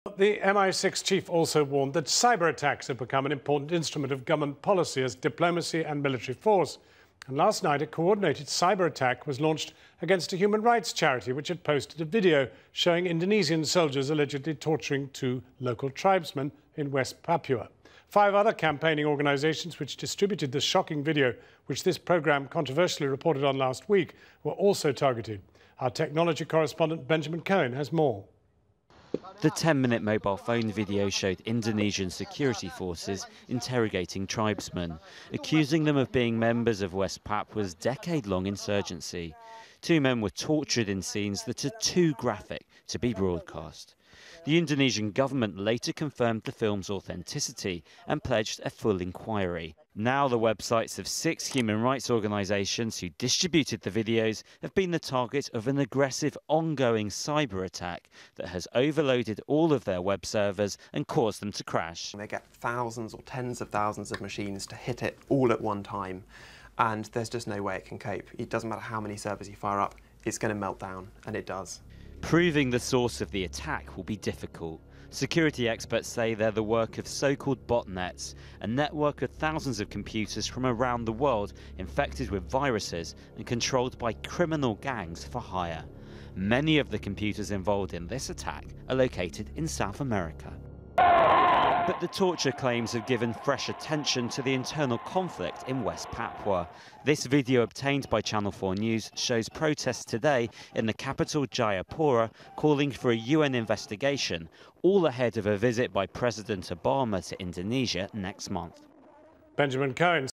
Cyber attack on charity 'directed by governments'
Several West Papuan charity human rights organisation websites raising Papua human rights abuses were crippled by a cyber attack.
It's clearly someone doesn't want the world to know/see the human rights abuse in Indonesia.
Most likely Indonesian authorities paid an computer expert/hacker to develop a program to bring down those sites which hosted the full 10min video of West Papuans being tortured.

All I can say is: SHAME ON YOU all those who try to cover it up.
The word is out and there's nothing that's going to stop the voice of West Papua.
PAPUA MERDEKA!

Video uploaded for the world to see.
Taken from channel4 UK.

The following organization websites are under attack:
Survival International
Friends of People Close To Nature
West Papua Media Alerts
Asian Human Rights Commission
Free West Papua Campaign
West Papua Unite